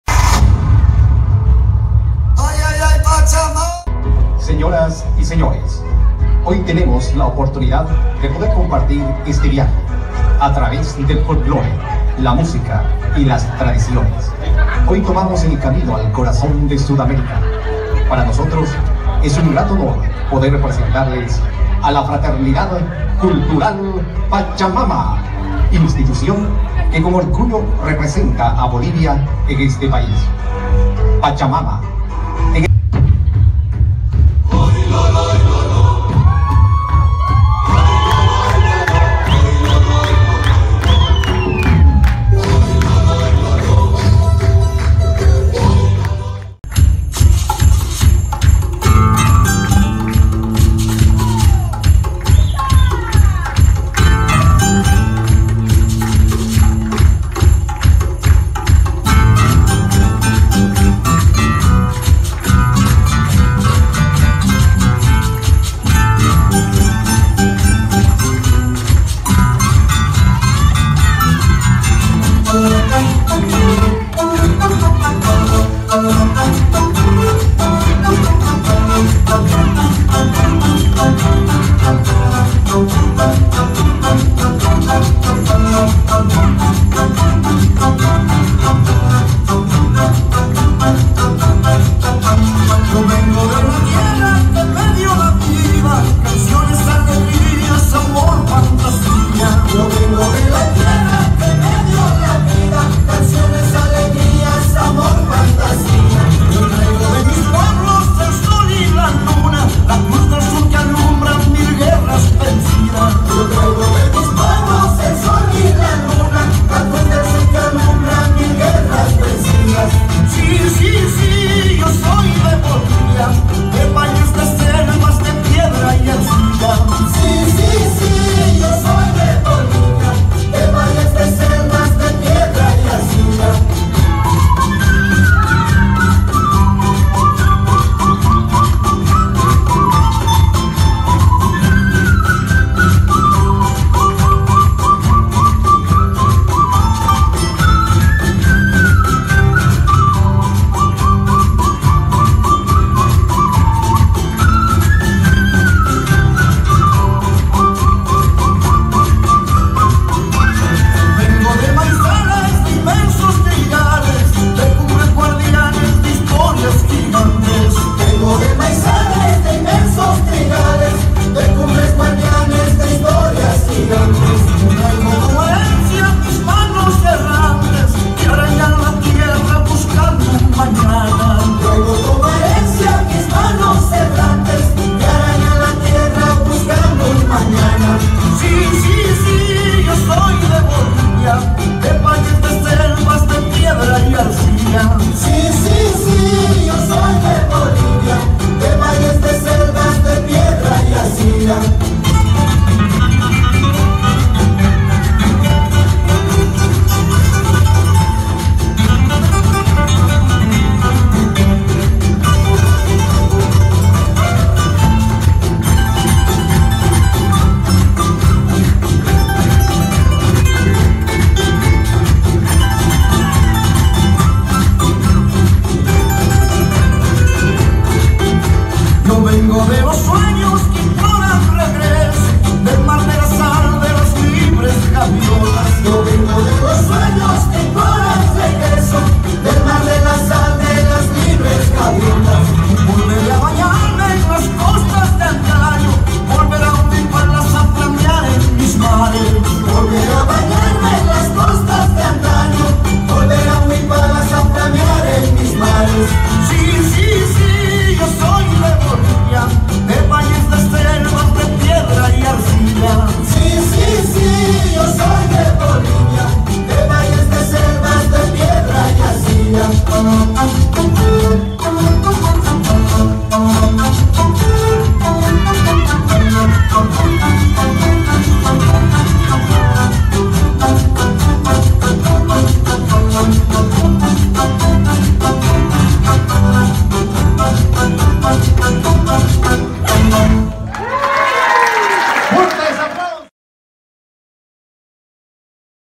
BALLET EFOBOL SAYA ENCUENTRO 2019 HD1080
Open house Fraternidad Cultural Pachamama, grupo invitado.